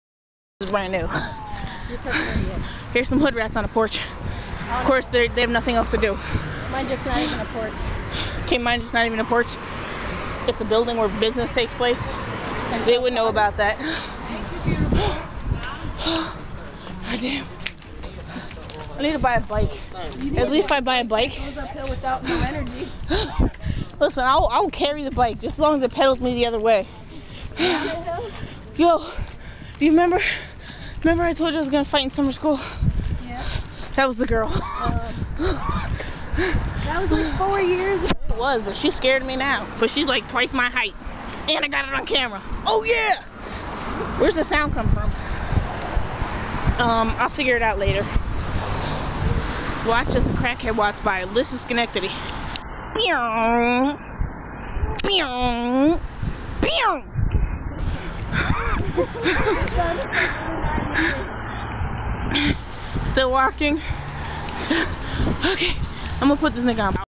schenectady you in?
me my cus and ann-bizzle walkin hallo